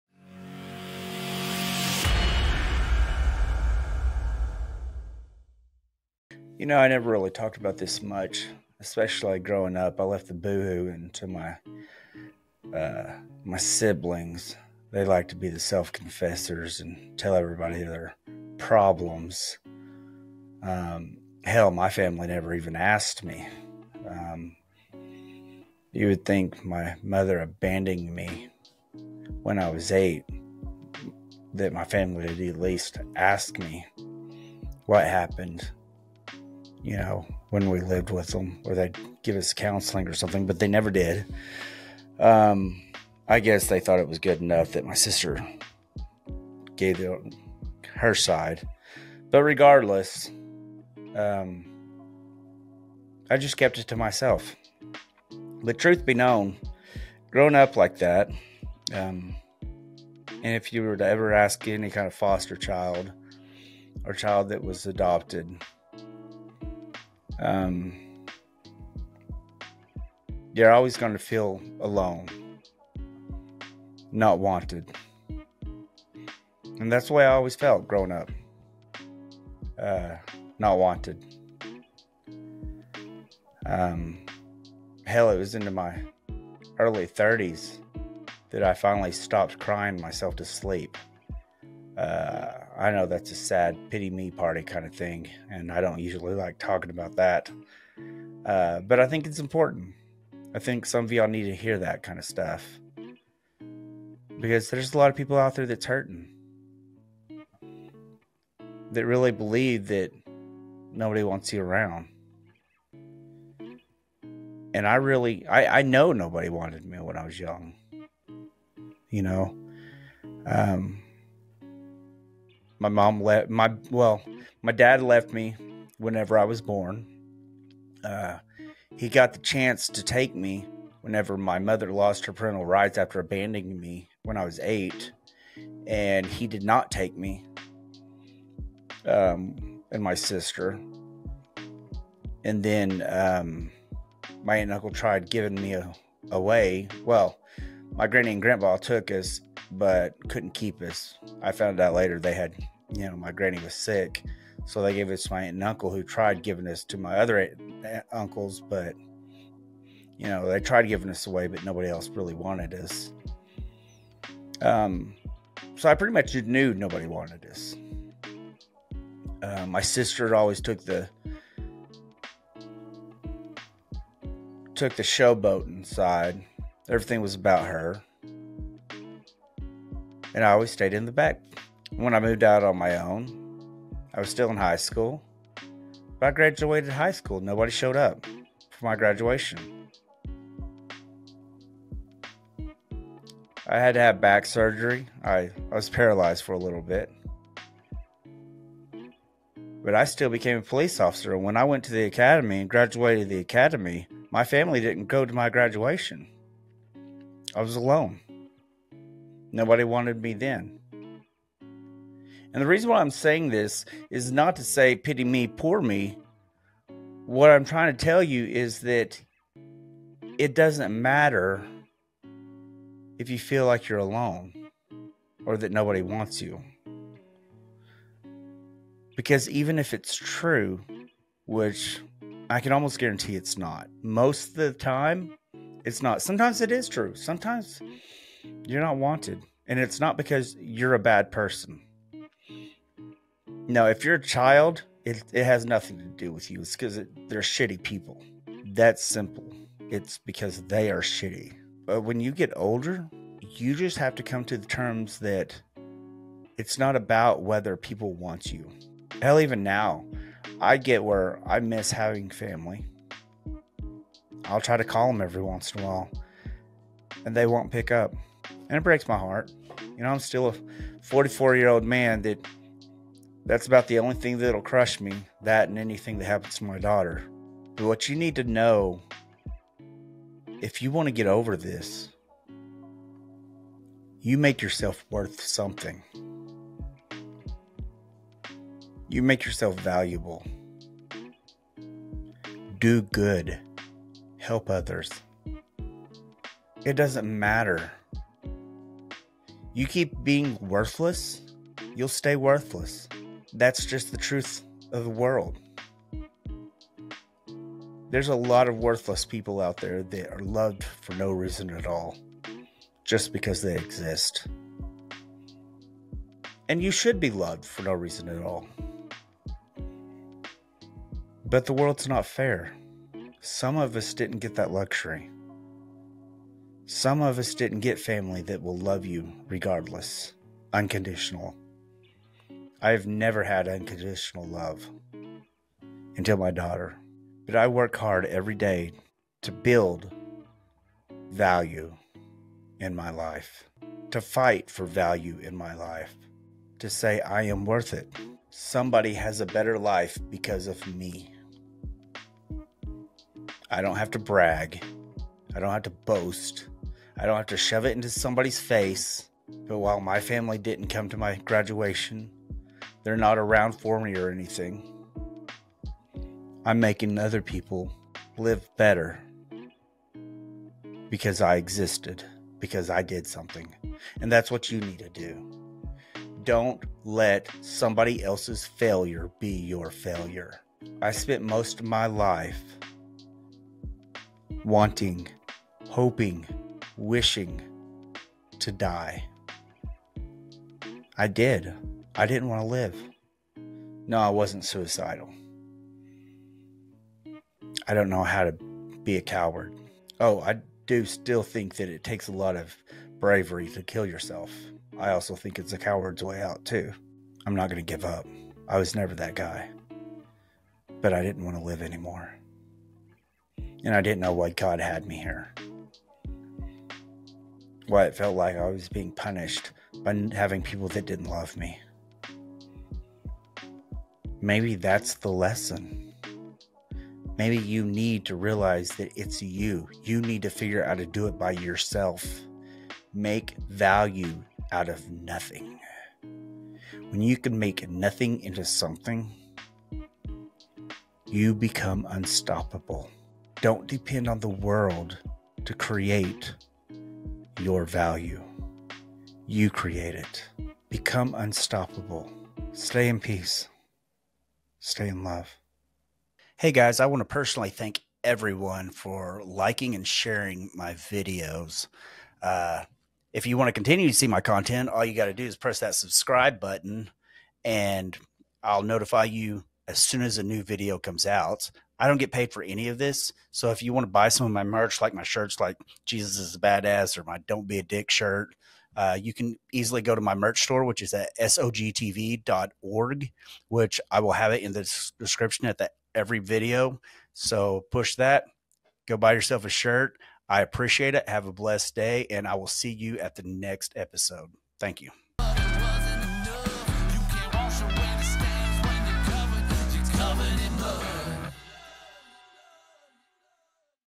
How to Build Value From The Struggle
Summary

In this conversation, Joshua Daniel Hayes opens up about his experiences of feeling alone and unwanted. He shares how his family never asked about his struggles and how he carried the burden of abandonment throughout his life. Despite the lack of support, Joshua emphasizes the importance of not letting others' actions define one's worth. He encourages listeners to create their own value and make a positive impact on others. Ultimately, Joshua highlights the significance of finding peace and love within oneself.

Takeaways

Feeling alone and unwanted can be a common experience for foster children or those who have been abandoned.
Lack of support from family can contribute to feelings of rejection and worthlessness.
It is important to recognize that one's worth is not determined by others' actions or opinions.
By creating value in one's own life and making a positive impact on others, it is possible to overcome feelings of loneliness and find peace and love within oneself.

$JoshuaDanielHayes

Chapters

00:00 Feeling Alone and Unwanted
03:05 Lack of Support from Family
04:03 Overcoming Feelings of Rejection
05:20 Creating Your Own Worth
07:42 Making a Positive Impact
09:03 Taking Control of Your Value
09:22 Finding Peace and Love